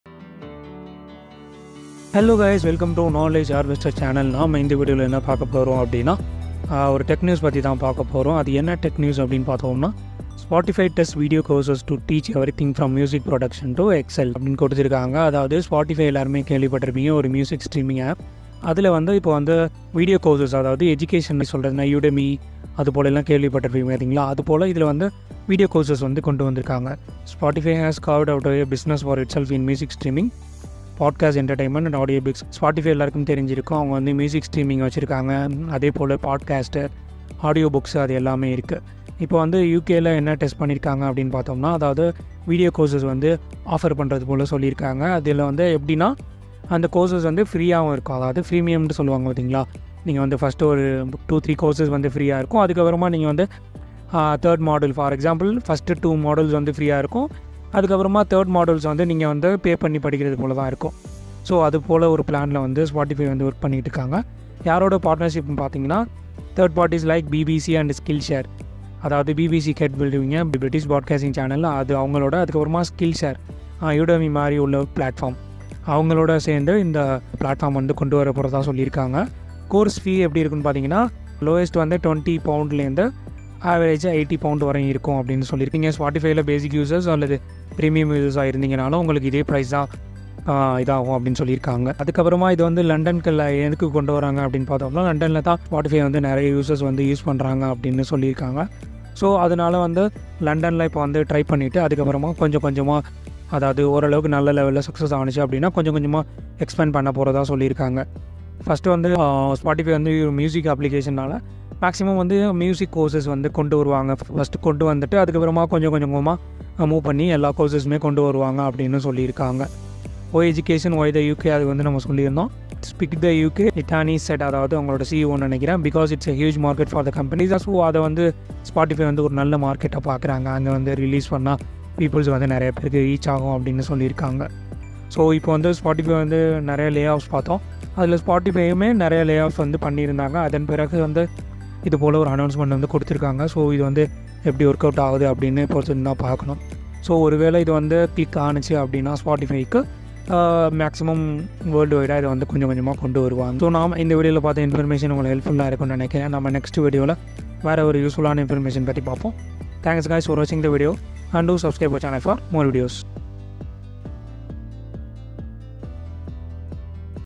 Spotify Testing Video Based Learning Courses
In today's video, we're diving into an exciting update from Spotify that's set to revolutionize the way we learn. Spotify, traditionally known for its vast music and podcast library, is now entering the educational sphere by introducing video courses. These aren't just any courses; they're offered on a freemium basis, making learning accessible to a broader audience. With prices ranging from £20 to £80, there's something for everyone. Starting with music courses, Spotify is testing the waters in the United Kingdom, hinting at a global expansion in the future.

Watch this video fully and ask any doubts in the comment section.